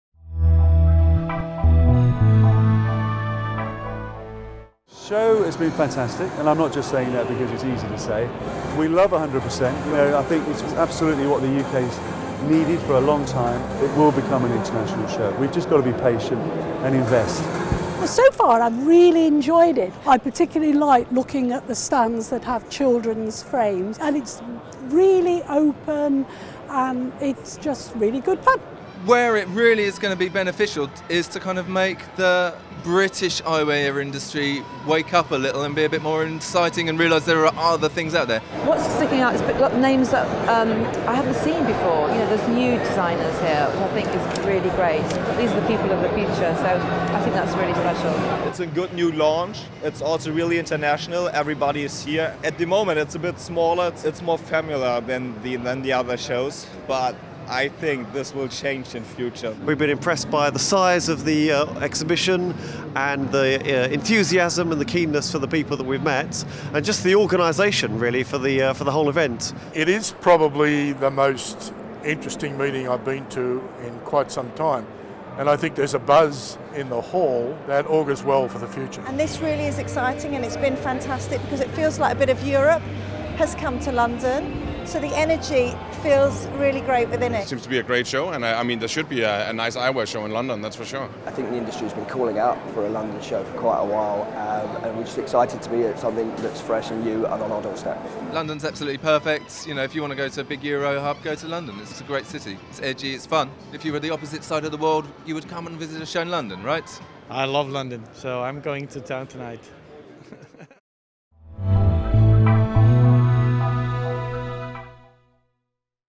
100% Optical review
OT finds out what 100% Optical delegates, exhibitors and speakers thought of the show